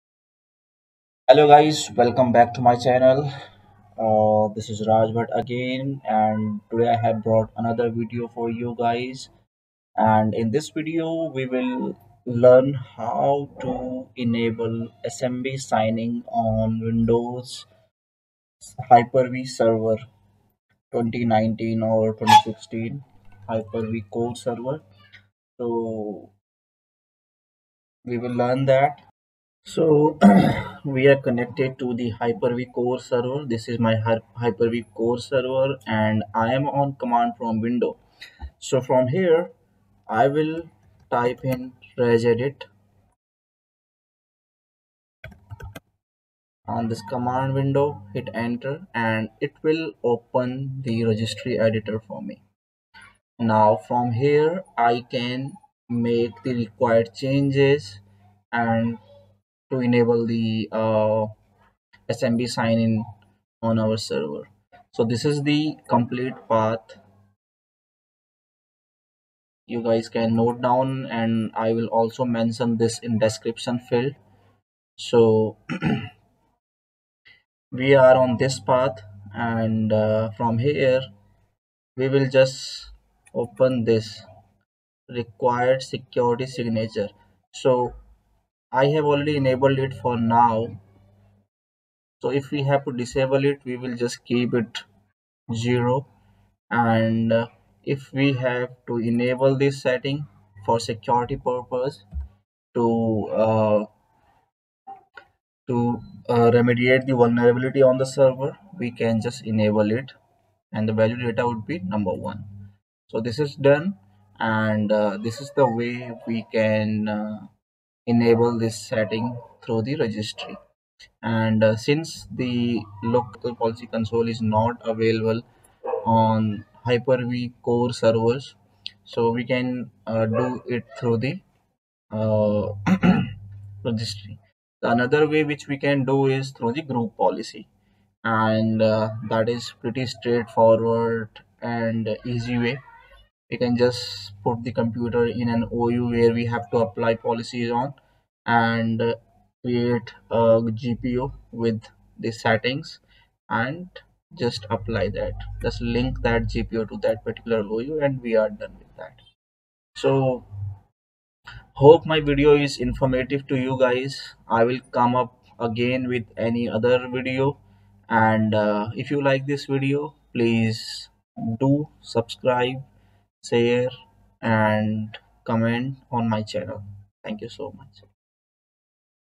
"Enable" 'Microsoft network server: Digitally sign communications (always)' on windows hyper-v core
This video helps to know, how to enable SMB Sign in on windows Hyper-V core servers, where we don't have local policy console.

Path- HKEY_LOCAL_MACHINE\SYSTEM\CurrentControlSet\Services\LanmanServer\Parameters